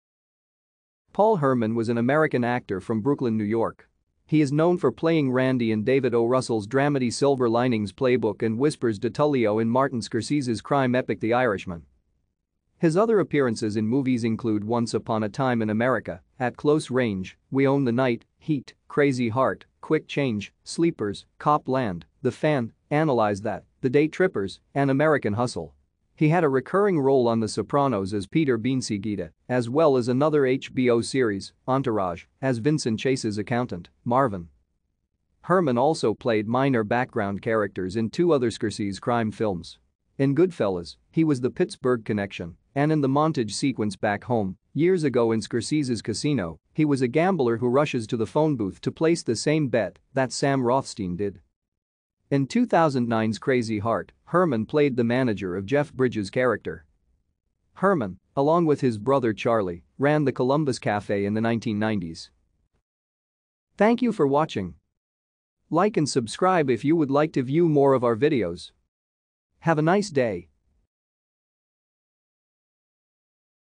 Paul Herman
Paul Herman  was an American actor from Brooklyn, New York. He is known for playing Randy in David O. Russell's dramedy Silver Linings Playbook  and Whispers DiTullio in Martin Scorsese's crime epic The Irishman.